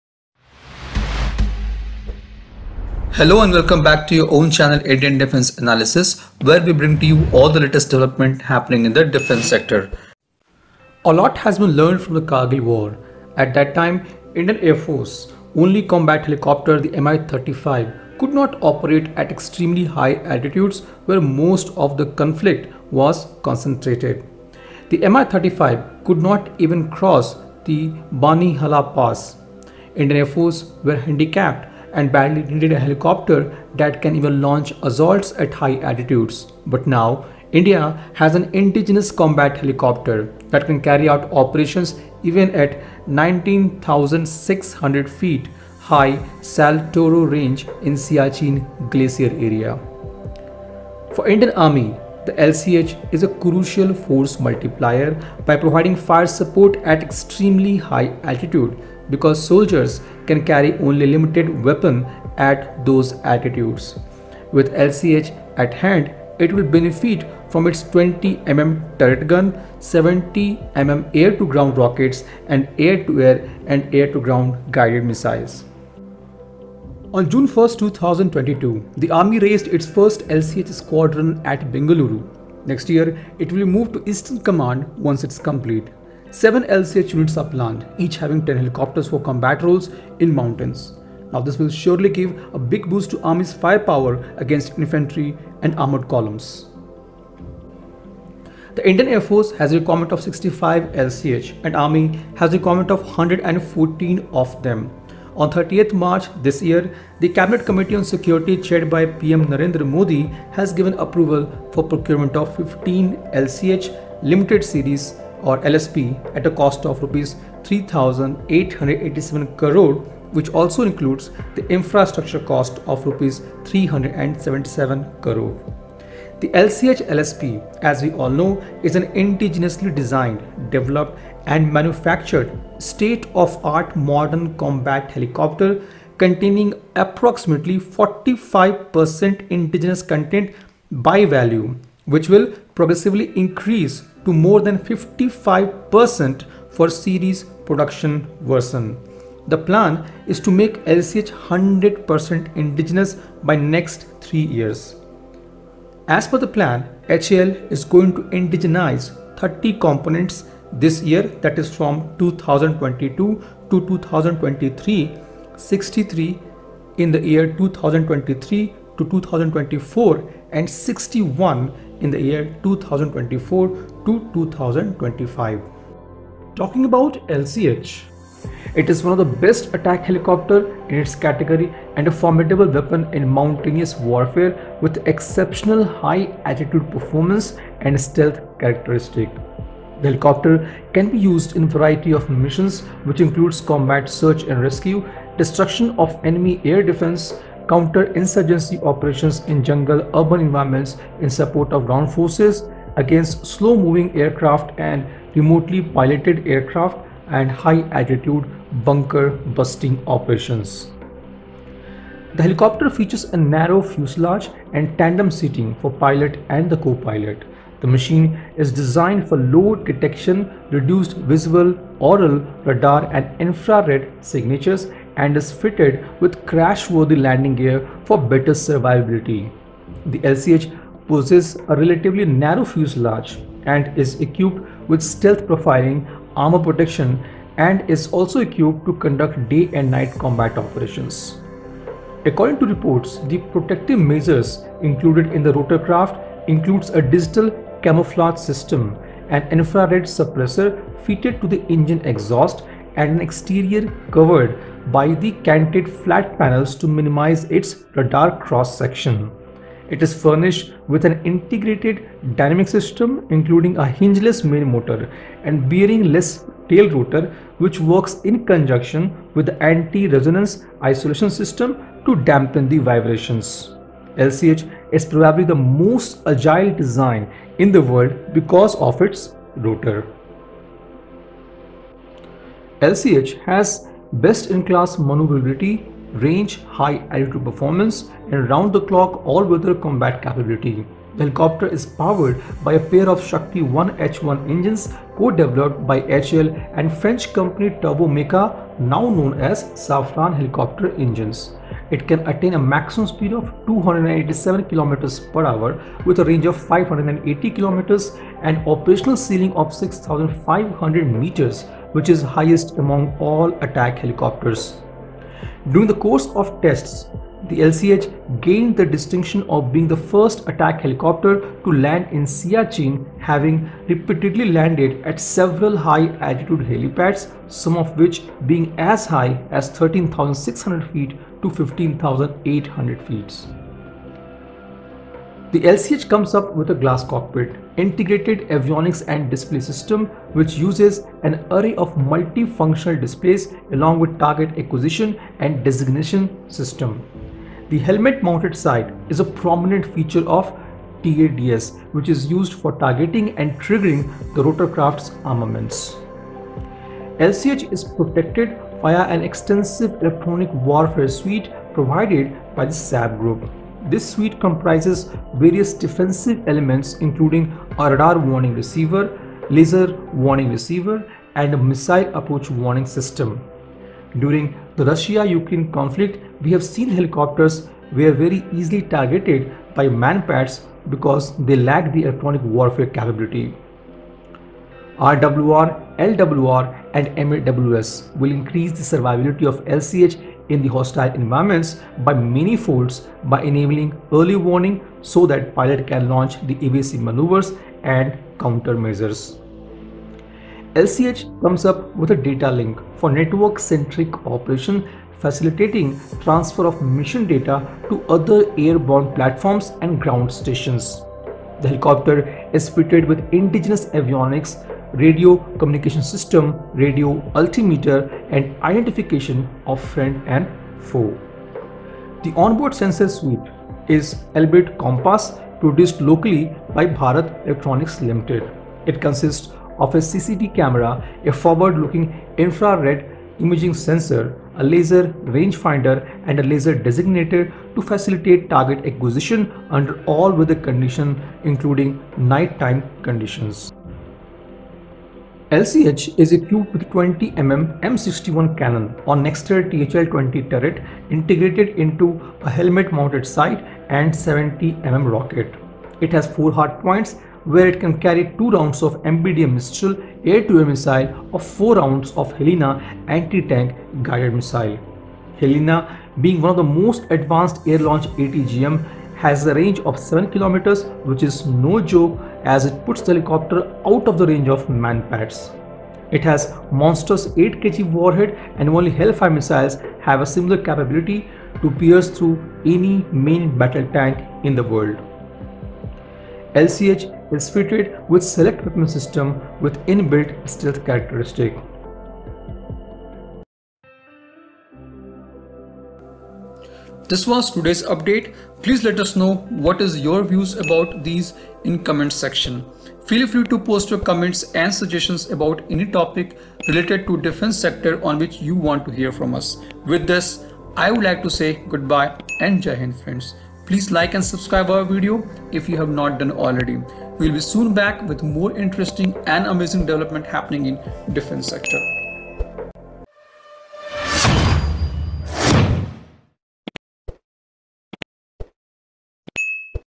1st Army Squadron of Light Combat Helicopter in Bengaluru
Need for LCH

A lot has been learnt from the Kargil war. At that time IAF's only combat helicopter, the Mi-35, couldn’t operate at extremely high altitudes where most of the conflict was concentrated. "The Mi-35 couldn’t even cross the Banihal pass. IAF were handicapped and badly needed a chopper that can even launch assaults at high altitudes. But now, India has an indigenous combat helicopter that can carry out operations even at the 19,600-feet high Saltoro Range in Siachen Glacier area.

Army’s 1st LCH Squadron

On June 1, 2022, the Army raised its first LCH squadron at Bengaluru. Next year, It will moved to Eastern Command once it’s complete. 7 LCH units are planned, each having 10 helicopters for combat roles in the mountains. This will give big boost to Army’s fire power against infantry and armoured columns.

CCS approves 15 LCH

The Indian Air Force has requirement of 65 LCH and army has requirement of 114 of them. On 30th March 2022, the Cabinet committee on security chaired by PM Narendra Modi has given approval for procurement of 15 LCH – light combat Helicopter limited series production or LSP at the cost of Rs 3,887 crore along with the infrastructure cost of Rs 377 crore. The LCH LSP as we all know is an indigenously designed, developed and manufactured state–of–the–art modern combat helicopter containing approximately 45% indigenous content by value which will progressively increase to more than 55% for series production version. The plan is to make LCH 100% indigenous by next 3 years. within next 3 years. As per plan HAL is going to indigenize 30 components in the year 2022-23, 63 in the year 2023-34 and 61 in the year 2024-25.

More Indigenization of LCH

Many critical components have already been indigenized and recently the helicopter division of HAL has released lot tenders for the participation of private firms in the indigenization of the many components as you can see on your screen.

The most important of them which have been indigenized is AFCS. The LCH had been flying with an expensive, imported AFCS or automatic flight control system (AFCS) which has been replaced by an indigenous one. AFCS is a powerful computer that keeps a helicopter flying stably, by sensing any deviation from level flight in microseconds, and sending flight controls the correctives needed to revert to stable flight.

HAL has also indigenized the Cockpit Display System on LCH with the active participation of private firms. The LCH is now furnished with a glass cockpit which accommodates an Integrated Avionics and Display System (IADS) which uses an array of multifunction displays along with the onboard target acquisition and designation (TADS) system. A prominent element of the TADS system is the helmet mounted sight (HMS), which serves as the principal instrument for targeting and triggering the rotorcraft's armaments.

Engine as always have been the most difficult and critical of all the technologies to be developed indigenously. HAL along with French Engine maker Safran has developed Ardiden 1H1 Shakti derived from Safran Ardiden turboshaft engines and fitted with infrared suppressors in the engine exhaust. The engine powers LCH as well as ALH Dhruv helicopters of HAL.

About LCH

It is one of the best attack helicopters in its category and a formidable weapon in mountain warfare with exceptional high-altitude performance and stealth characteristics. The helicopter can be used in variety of mission which includes Combat Search and Rescue (CSAR), Destruction of Enemy Air Defence (DEAD), counter–insurgency (CI) operations in jungle and urban environments in support to ground forces, against slow moving aircraft and Remotely Piloted Aircraft (RPAs), high–altitude bunker busting operations